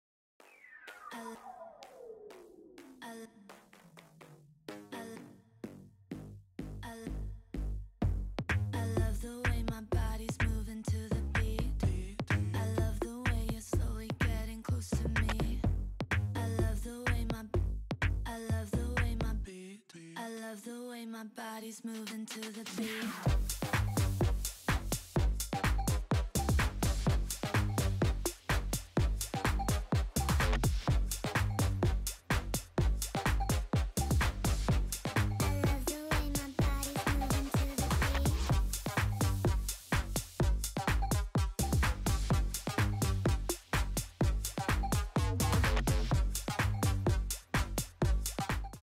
Model Sophie Linnea Poses backstage at Artheartsfashion NYFW
4K/60/10 Bit/n-log/Nikon Z9
IG nyftimes
IG artheartsfashion
IG sophielinneaa